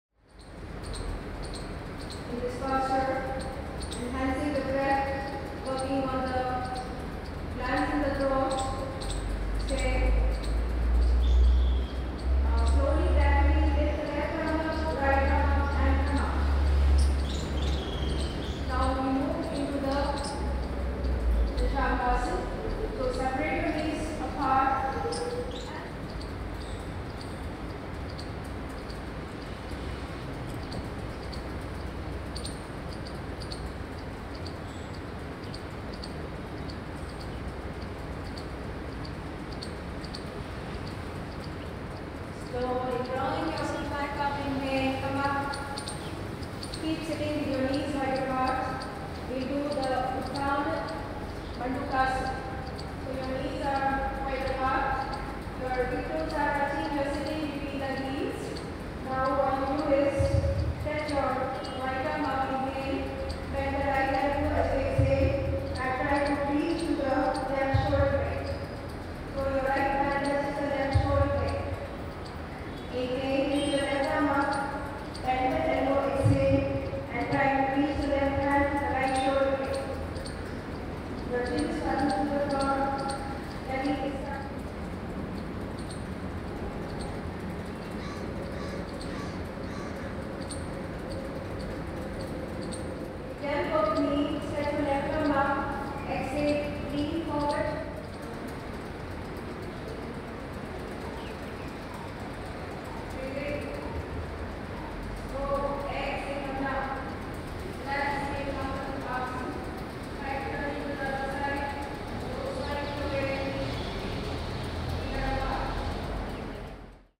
Yoga asanas for neck and back pain | Shivananda's Yogalife
Delhi Rotary club recently collaborated with Shivananda's Yogalife centre for the International Day of Yoga celebrations on 21st June 2019. Yoga instructor, Smiti of Yogalife, took charge of the session towards a better India in terms of fitness and mental health.